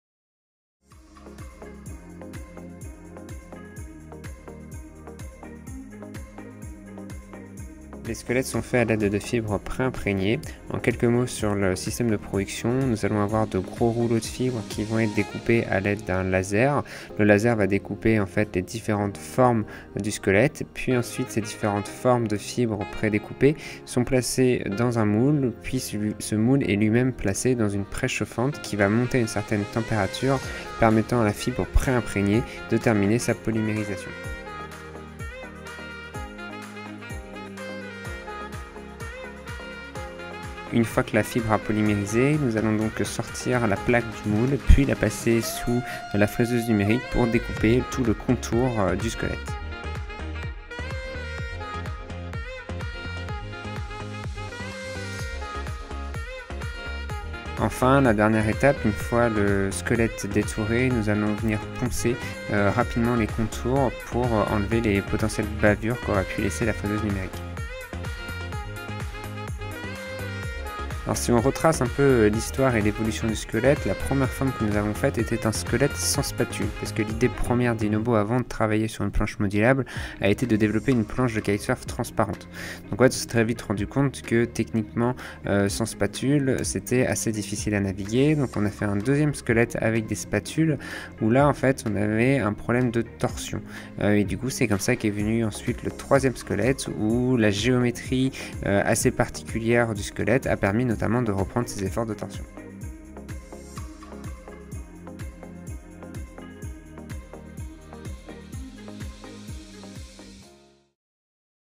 [INOBO] The skeleton
INOBO is not just a « board », it’s a combination of a carbon exoskeleton and a deck. We completely change the structure of a classic kiteboard, and nobody has done that before. On one hand, the carbon exoskeleton ensures stiffness to the board, whereas on the other hand, the deck enables you to go from a comfy, steady board to a playfull and dynamic one depending on the deck you choose. The range of decks we suggest varies on size, shape and material (transparent, wood or carbon) and by combining one to the exoskeleton, you are shaping your board as you like it to be.